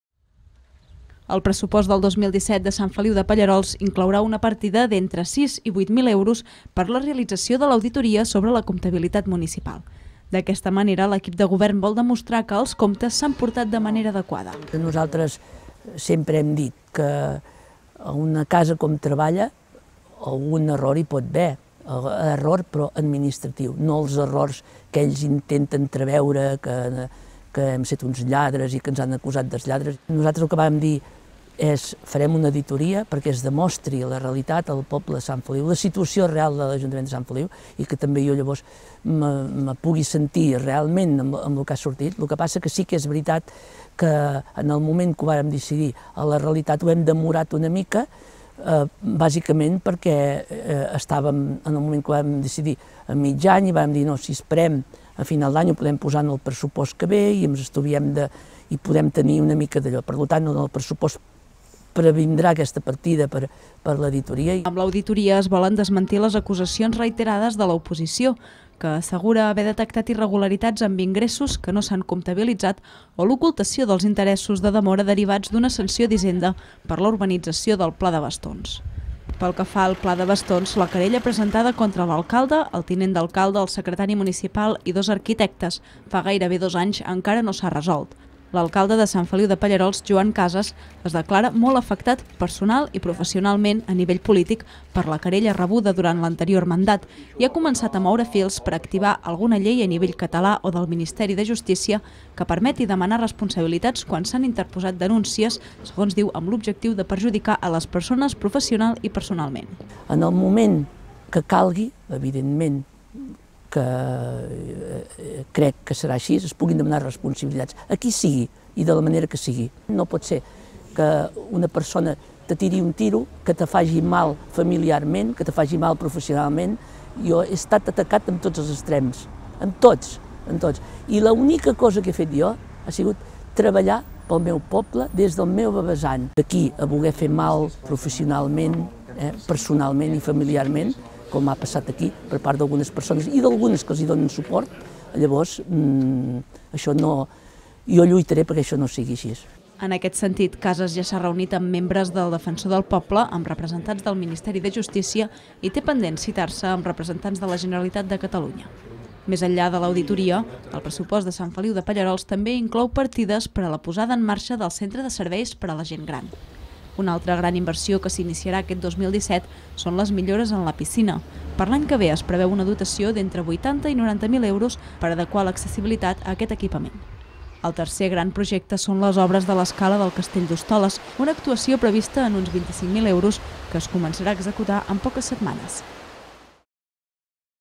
El pressupost pel 2017 de Sant Feliu de Pallerols inclourà una partida per a l’auditoria
A mitjans d’aquest any l’equip de govern de Sant Feliu de Pallerols va anunciar que encarregaria una auditoria per demostrar que els comptes de l’Ajuntament s’han portat de forma adequada. Ara aquesta auditoria compta amb una partida d’entre 6.000 i 8.000 euros en els pressupostos que es preparen per al 2017 i que s’aprovaran a finals de mes. L’any que ve també es tirarà endavant la reforma del Centre de Serveis per a la Gent Gran i es faran millores a la piscina i al Castell d’Hostoles.